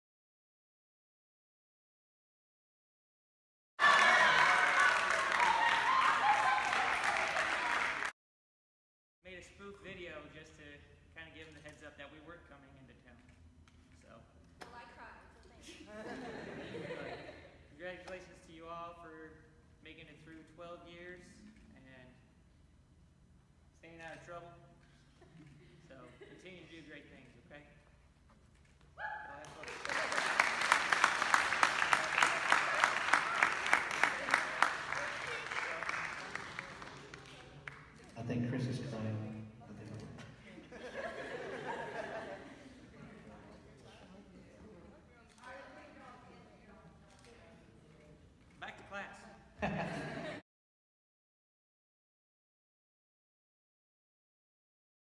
Parents surprise Cuero grad
Cuero High School senior Chris Calzada has not seen his parents in four years. The couple, U.S. Army Staff Sgt. Jarod Papciak and Ruby Papciak, surprised their son the week of his high school graduation at school.